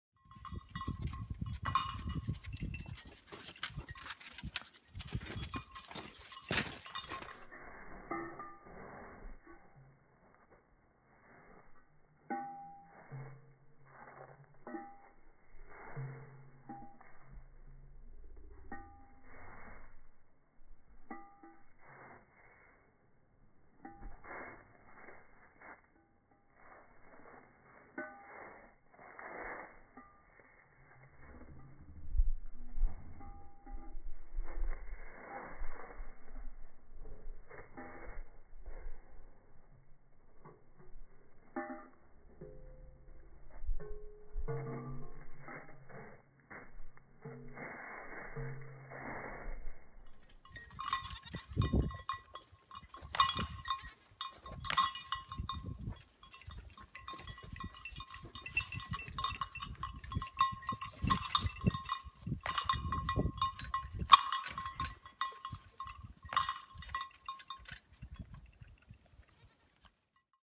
Baby goats of Snow Lake Keep
Watching our goats in pasture at Snow Lake Keep, Nova Scotia in early spring of 2017.